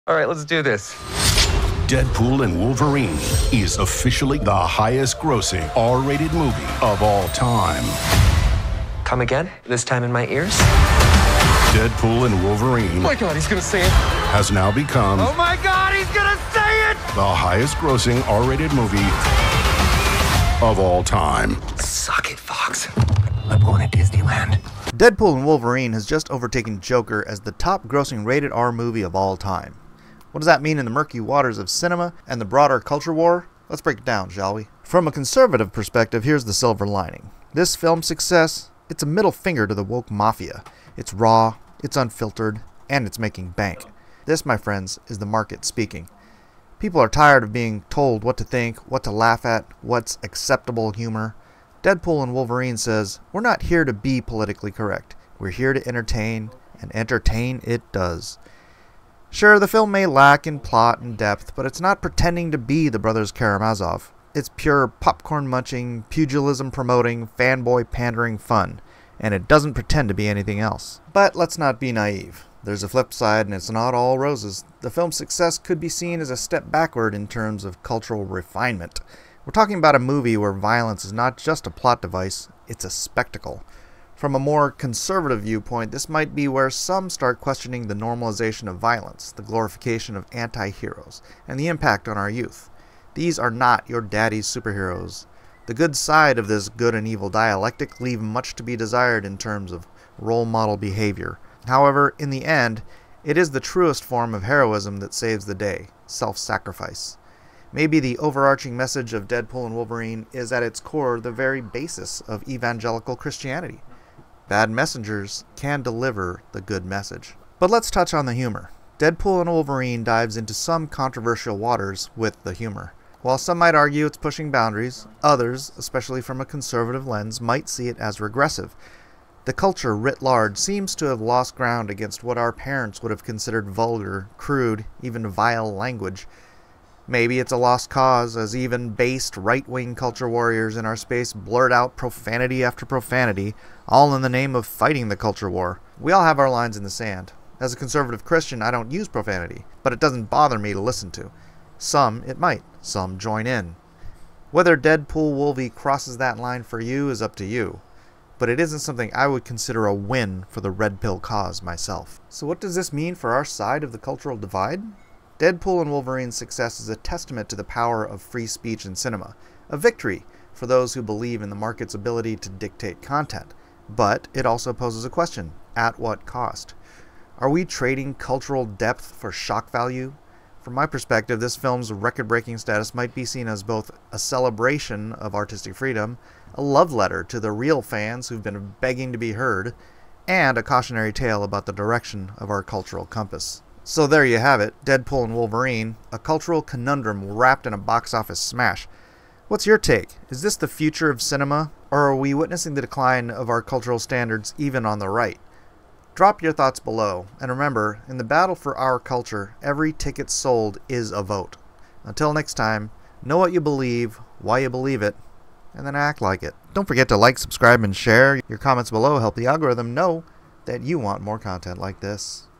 Deadpool & Wolverine Breaks Records for Rated R Movies! Culture Shock, or Pandering Schlock?
deadpoolandwolverinemovie deadpool deadpoolandwolverinetrailer wolverine This isn't the movie you think it is... or is it?

Does Deadpool and Wolverine subvert expectation, culture, class or norms? Does it challenge the status quo or just spoon feed fandom what they've been begging for? Are we so hungry for something that doesn't try to jam woke agenda down our throats that we will lower our standards and watch just about anything that makes us giggle?  You tell me.